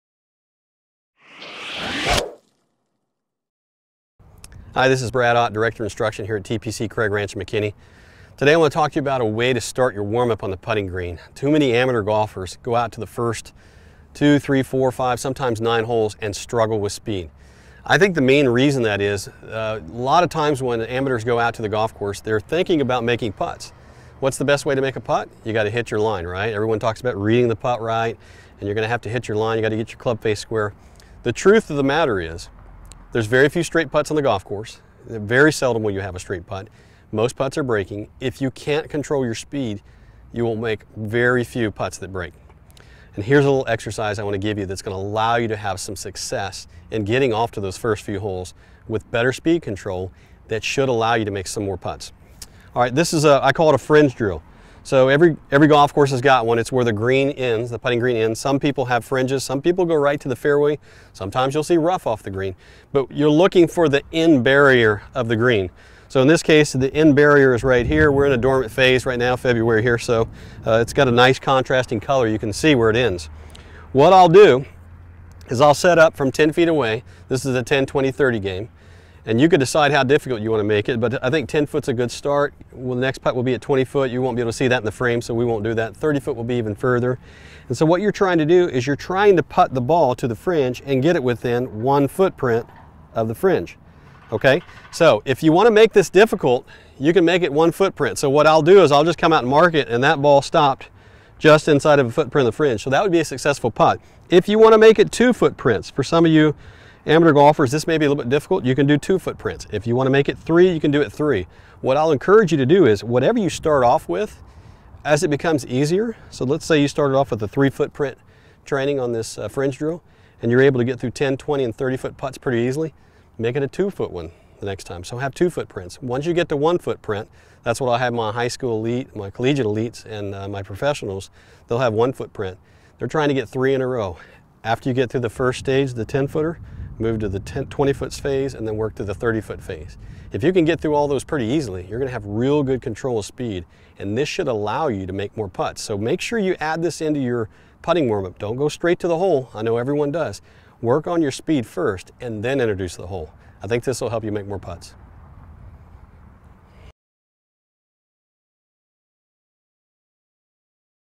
Doing This As A Warm Up Drill Will Improve Your Golf Putting
Watch "Doing This As A Warm Up Drill Will Improve Your Golf Putting" presented by Brad Ott from GolfSwing.com